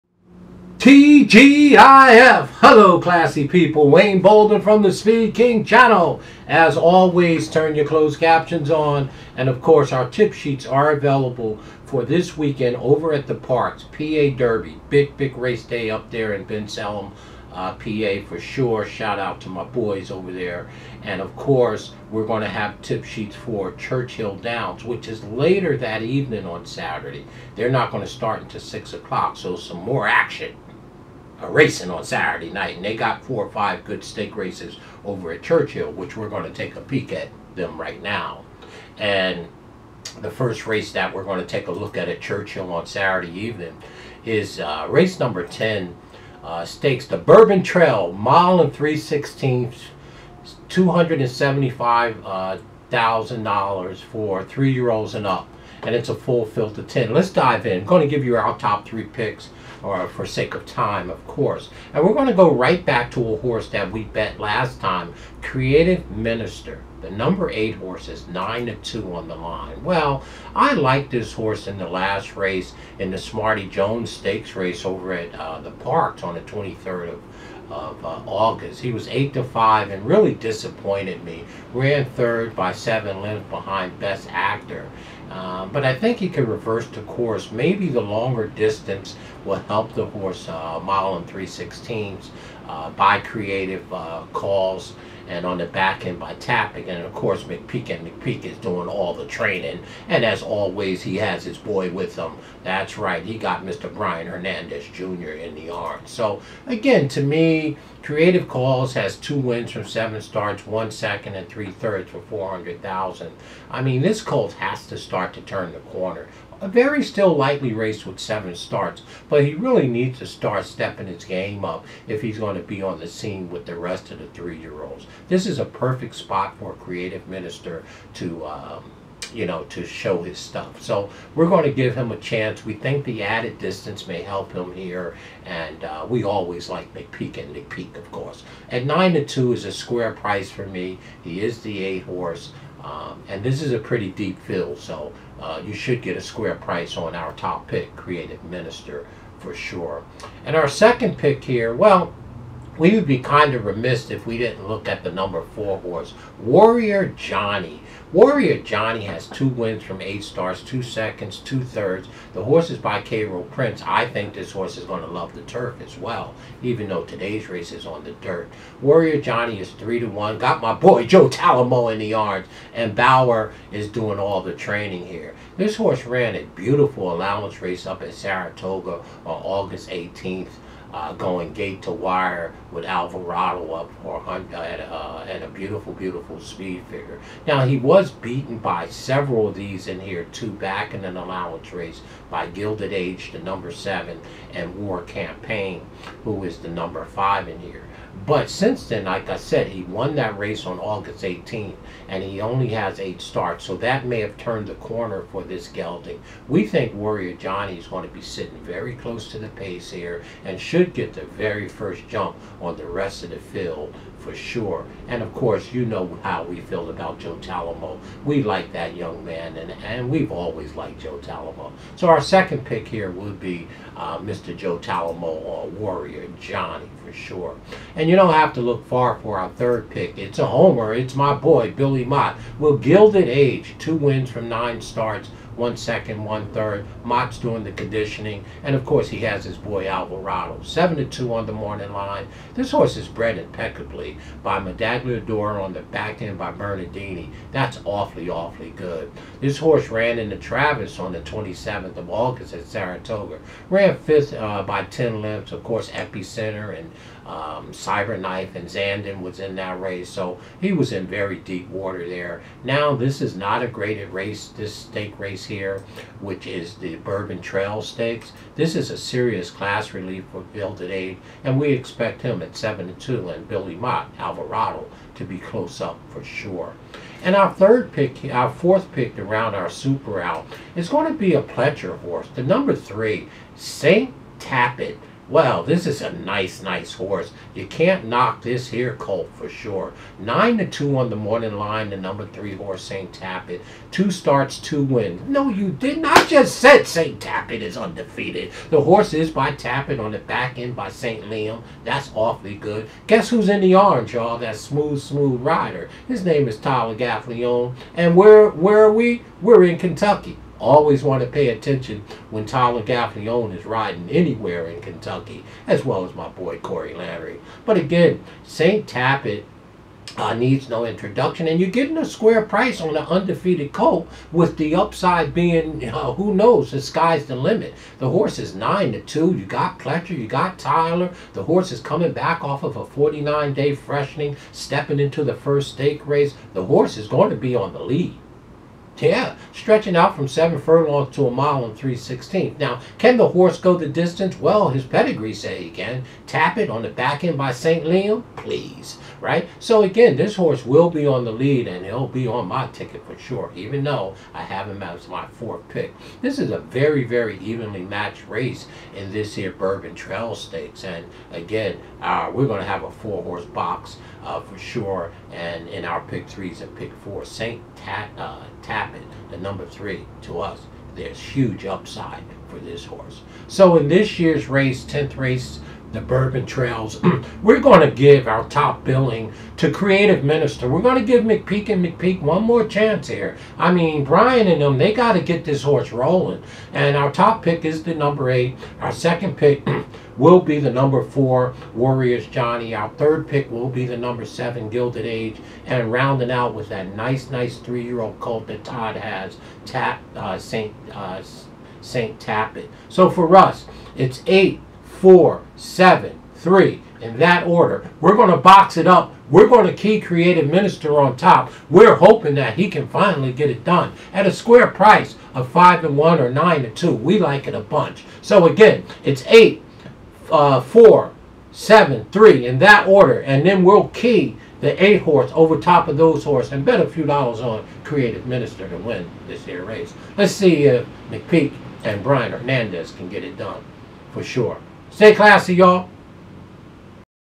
Saturday @Churchill "Bourbon Trail Stakes" Preview & Picks | 10th Race 9/24/2022!
Speed King's Lands On (#8 Creative Cause 9/2) W/ #4,#7,#2
Opening Day At The Saratoga Race Courses